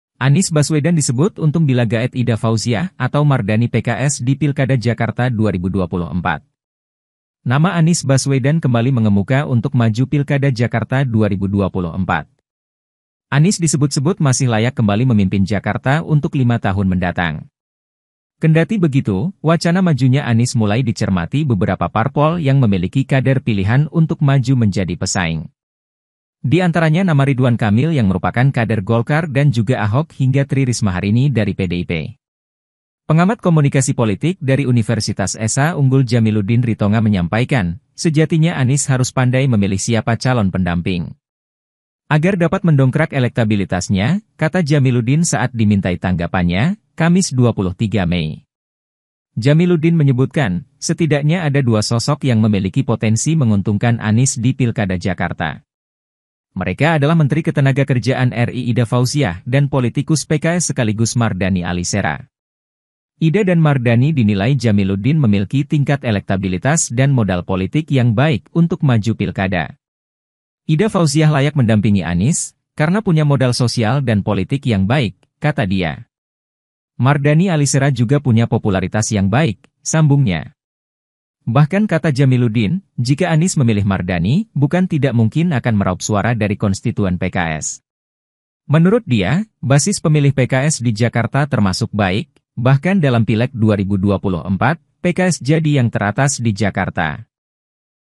Anies Baswedan Disebut Untung Bila Gaet Ida Fauziah Atau Mardani PKS di Pilkada Jakarta 2024
TRIBUNNEWS.COM, JAKARTA - Nama mantan Gubernur DKI Jakarta Anies Baswedan kembali mengemuka untuk maju dalam Pilkada Jakarta 2024.

Anies Baswedan disebut-sebut masih layak kembali memimpin Jakarta untuk lima tahun mendatang.

Kendati begitu, wacana kembali majunya Anies Baswedan mulai dicermati beberapa partai politik yang memiliki kader pilihan untuk maju menjadi pesaing.

Di antaranya nama Ridwan Kamil yang merupakan kader Partai Golkar dan juga Basuki Tjahaja Purnama alias Ahok hingga Tri Rismaharini dari PDIP.

Terkait hal tersebut, Pengamat Komunikasi Politik dari Universitas Esa Unggul Jamiluddin Ritonga menyampaikan, sejatinya Anies harus pandai memilih siapa calon pendamping jika ingin tetap maju dan menang di Pilkada Jakarta.

"Jadi, Anies meski punya pasangan yang cukup baik agar dapat mendongkrak elektabilitasnya," kata Jamiluddin saat dimintai tanggapannya, Kamis (23/5/2024).

Jamiluddin menyebutkan, setidaknya ada dua sosok yang memiliki potensi menguntungkan Anies di Pilkada Jakarta.

Mereka adalah Wakil Ketua Umum PKB sekaligus Menteri Ketenagakerjaan RI Ida Fauziah dan Politikus PKS sekaligus Anggota Komisi II DPR RI Mardani Ali Sera.

Ida Fauziah dan Mardani Ali Sera dinilai Jamiluddin memilki tingkat elektabilitas dan modal politik yang baik untuk maju di Pilkada.

"Ida Fauziah layak mendampingi Anies, karena punya modal sosial dan politik yang baik. Karena itu, Ida berpeluang dapat meningkatkan elektabilitas dirinya maupun Anies," kata dia.

"Mardani Ali Sera juga punya popularitas yang baik," sambungnya.

Bahkan kata Jamiluddin, jika Anies memilih Mardani maju di Pilkada Jakarta, bukan tidak mungkin akan meraup suara dari konstituen PKS.

Menurut dia, basis pemilih PKS di Jakarta termasuk baik, bahkan dalam Pileg 2024, PKS menjadi yang teratas di Jakarta.

"Karena itu, Mardani juga potensial mendongkrak elektabilitas dirinya dan Anies. Hanya saja, modal ekonomi Mardani tampaknya tidak sebaik Ida Fauziah," ucap dia.

Atas hal itu, Jamiluddin berpandangan, Anies Baswedan harus lebih pandai dalam memilih pasangan yang baik jika ingin kembali maju sebagai calon gubernur dalam Pilkada Jakarta 2024.

Terlebih untuk menggandeng PKB atau PKS, pasalnya, Anies tidak memiliki partai politik dan sangat berpotensi tidak bisa maju jika tidak mendapatkan dukungan dari partai.

"Karena itu, Anies harus dapat tetap menjaga hubungan minimal dengan Nasdem, PKB, dan PKB agar peluangnya maju tetap mendapat dukungan dari tiga partai tersebut," ucap Jamiluddin.